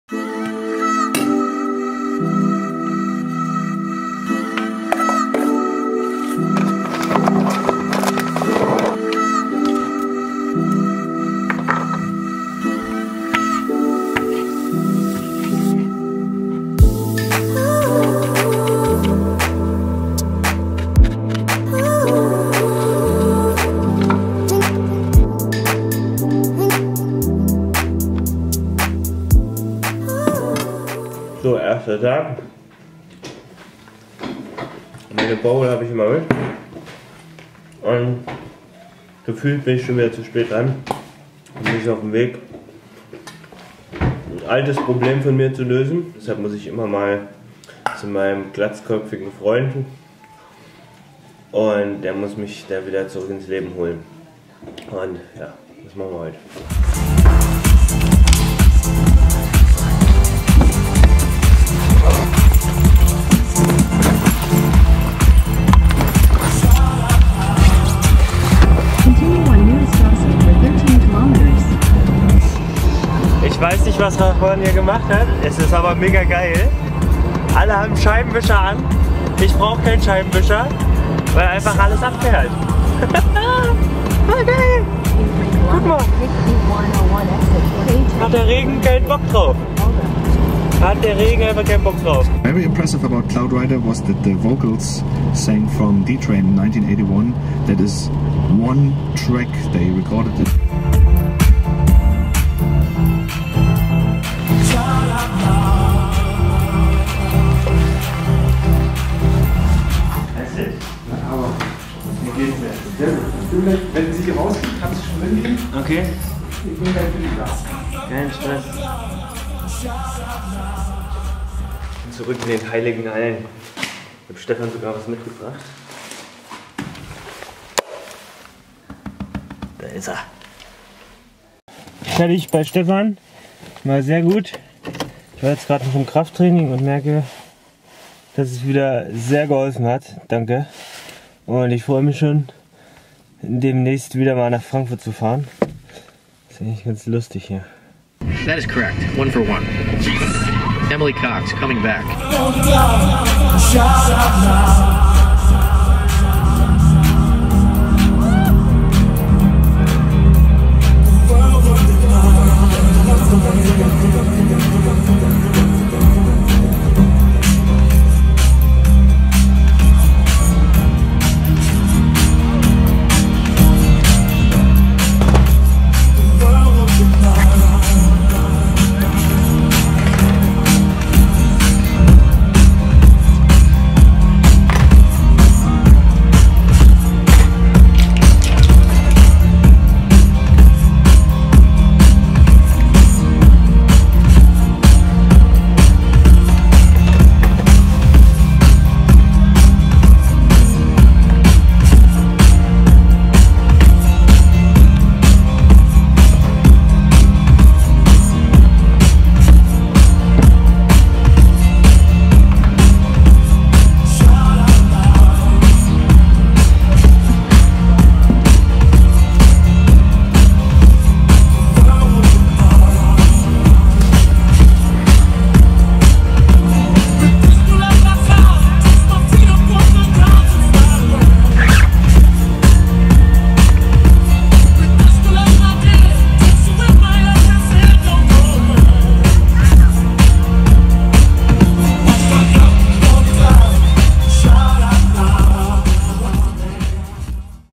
Ohne meinen Physio geht nichts
vlog79: Mein Physio rettet mir mal wieder das Leben!

Lasst mich was wissen was ihr schlecht und was ihr gut findet! Über ein Abo oder Like freu ich mich Besonders!